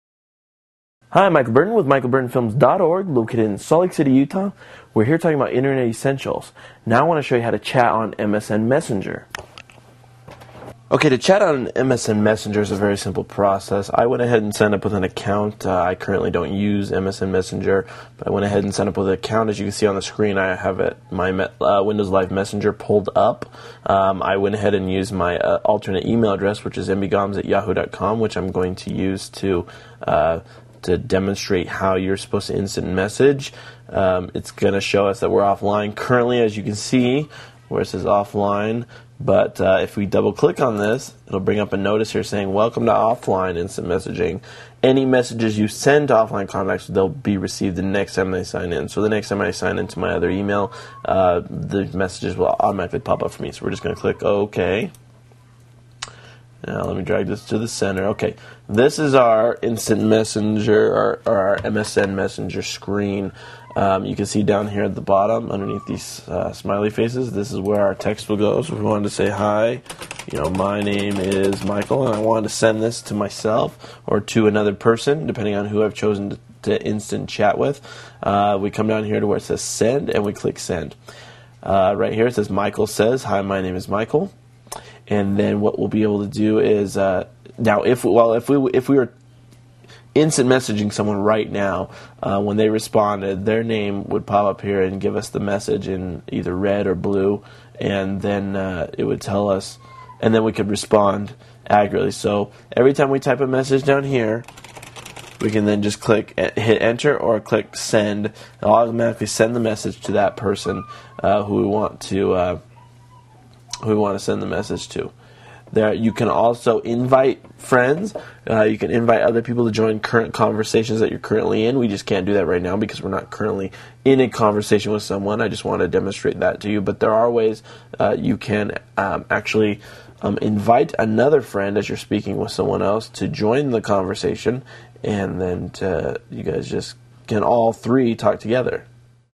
How to Chat on MSN Messenger
How to Chat on MSN Messenger. Part of the series: Internet Guide. To chat using MSN messenger, set up an account with an e-mail address and a password, sign in to the program, add the addresses of the people to chat with, then click on the chat button to start a conversation. Communicate with friends and family over the Internet by chatting with them via MSN messenger after watching this free video tutorial on social networking from an Internet professional.